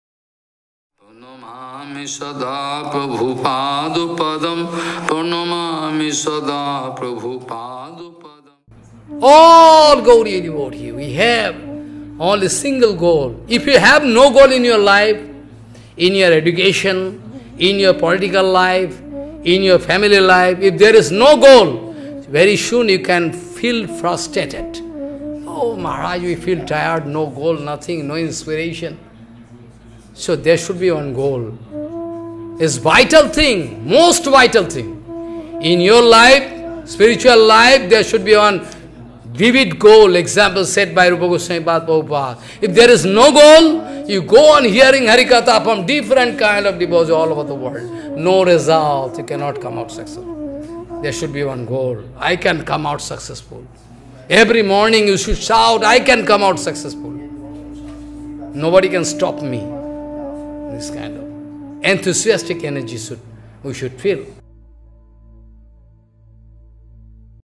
All Gaudiya Devotees we have only single goal. What is that goal? Excerpts from Shyam Baba's classes
All Gaudiya Devotees we have only single goal. What is that goal?  From From Brihad Bhagavatamrita 78 @BhaktiSiddhanta.org ​